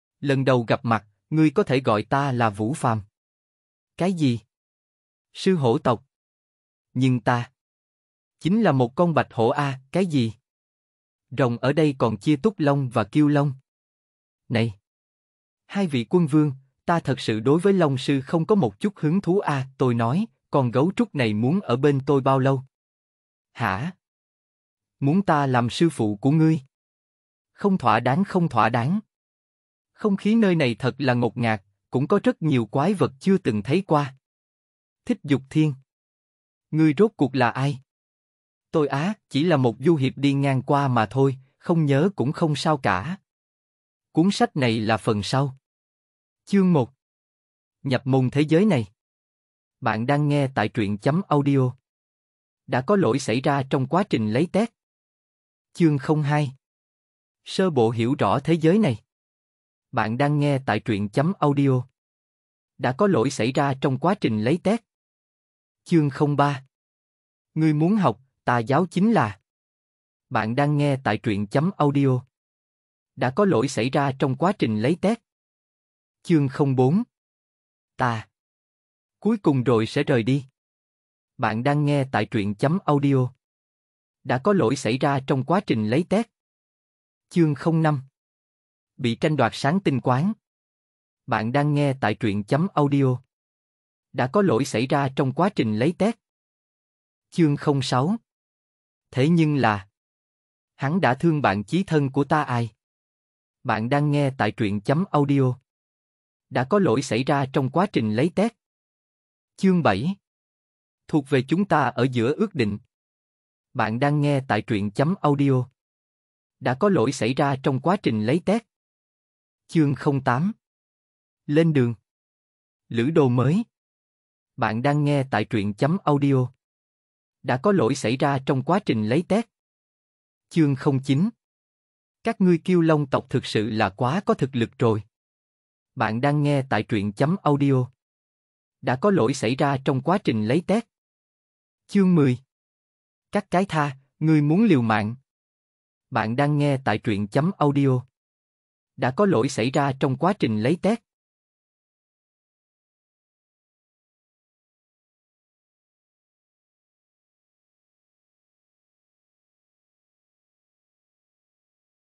Toàn Bộ Thú Xuất Kích Chi Vô Địch Nhưng Chỉ Muốn Điệu Thấp Chương 1-10 FULL | Dẫn Xuất Đồng Nhân, Nh
- w - đọc truyện chữ + audio

00:00:00 Chương 1:: Nhập môn thế giới này
00:00:55 Chương 02:: Sơ bộ hiểu rõ thế giới này
00:01:05 Chương 03:: Ngươi muốn học, ta giáo chính là
00:01:16 Chương 04:: Ta... Cuối cùng rồi sẽ rời đi
00:01:27 Chương 05:: Bị tranh đoạt sáng tinh quáng
00:01:36 Chương 06:: Thế nhưng là... Hắn đả thương bạn chí thân của ta ài
00:01:49 Chương 7:: Thuộc về chúng ta ở giữa ước định
00:01:59 Chương 08:: Lên đường! Lữ đồ mới!
00:02:10 Chương 09:: Các ngươi kiêu long tộc thực sự là quá có thực lực rồi!
00:02:21 Chương 10:: Cắt cái tha, ngươi muốn liều mạng?

Intro:

Lần đầu gặp mặt, ngươi có thể gọi ta là Vũ Phàm. Cái gì? Sư Hổ tộc? Nhưng ta. chính là một con bạch hổ a?

Cái gì? Rồng ở đây còn chia Túc Long và Kiêu Long? Này? Hai vị quân vương, ta thật sự đối với Long sư không có một chút hứng thú a!

Tôi nói, con gấu trúc này muốn ở bên tôi bao lâu? Hả? Muốn ta làm sư phụ của ngươi? Không thỏa đáng không thỏa đáng!

Không khí nơi này thật là ngột ngạt, cũng có rất nhiều quái vật chưa từng thấy qua.

Thích Dục Thiên: Ngươi rốt cuộc là ai?

Tôi á? Chỉ là một du hiệp đi ngang qua mà thôi, không nhớ cũng không sao cả.

(Cuốn sách này là phần sau)

Bạn đang nghe Toàn Bộ Thú Xuất Kích Chi Vô Địch Nhưng Chỉ Muốn Điệu Thấp Tập 1 chương 1 đến 10